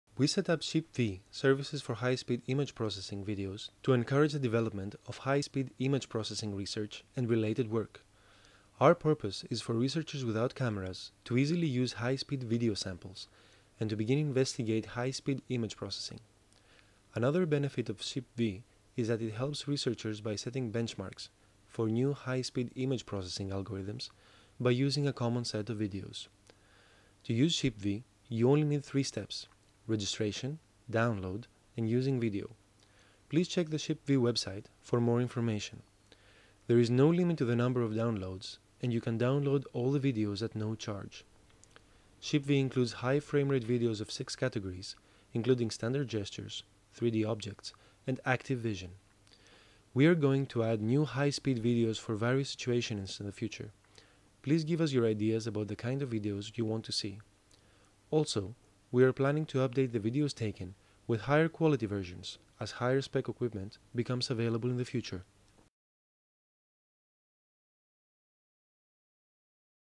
SHIP-v: High Frame Rate Video Samples for researches of High-speed Image Processing
Ishikawa Watanabe Laboratory starts SHIP-v (Services for High-speed Image Processing – videos) for research and development of high-speed image processing and related technology, so that researchers can easily utilize free video samples for their researches.
The high-speed image processing technology with enough frame rates to cover a necessary dynamics makes image processing so simple and easy to design effective algorisms and will pave the way to a number of new applications.
We are planning to add new videos for various situations and videos taken by higher-spec cameras in the future.